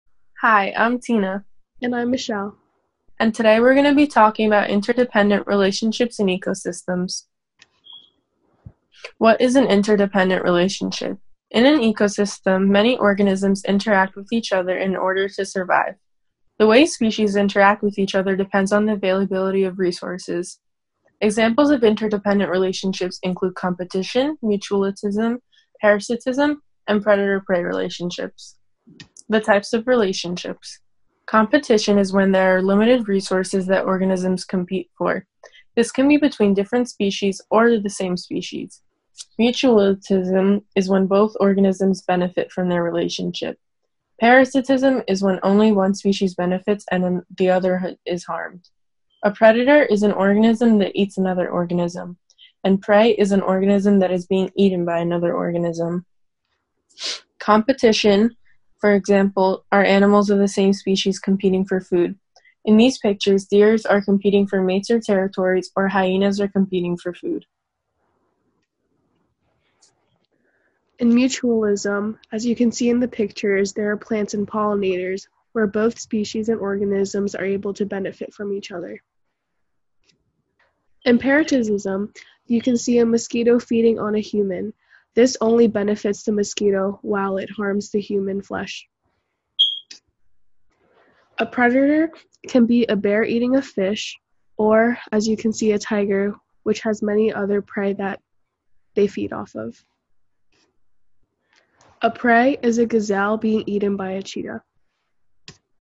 Interdependent Relationships In Ecosystems (Biology)
An explanation that predicts patterns of interactions among organisms in a variety of ecosystems. Emphasis is on predicting patterns of interactions such as competition, predation, mutualism, and parasitism in different ecosystems in terms of the relationships among and between organisms.
Links:
Worksheet: Answer key is on the second page of the document)